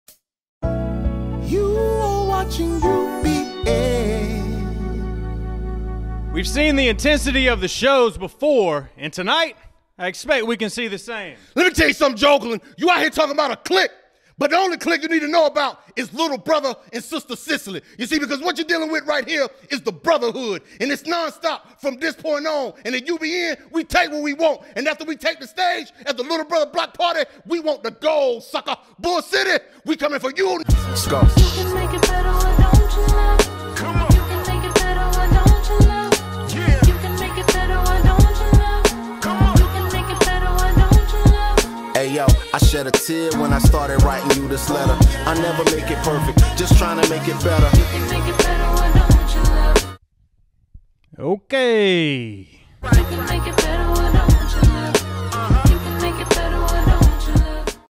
MADE IN DURHAM: A Little Brother Block Party (Promo)
MADE IN DURHAM: A Little Brother Block Party. 10.7.23.

Little Brother
Big K.R.I.T.
The Cool Kids
Zo! & Tall Black Guy

Sounds by:
Hourglass
Wally Sparks

Hosted by:
Sam Jay

Shoutout to the Durham Bulls for the locker room and the Harlem Heat for the inspiration.